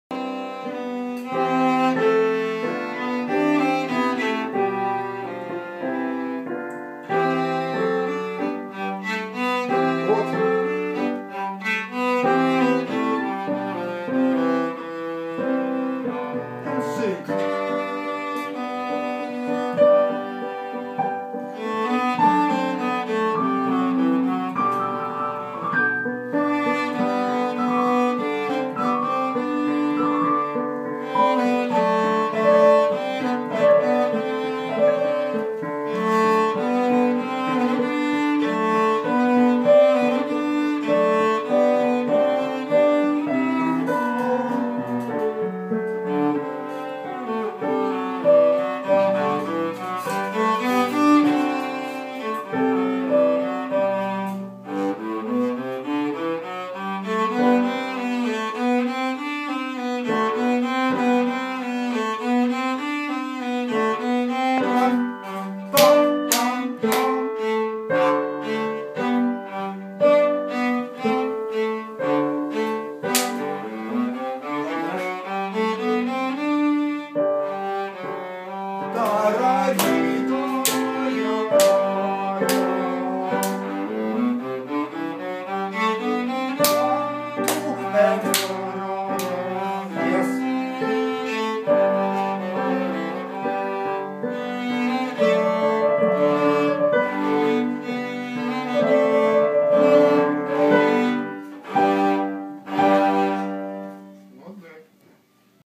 Not yet! A week before the recital
Ace playing the cello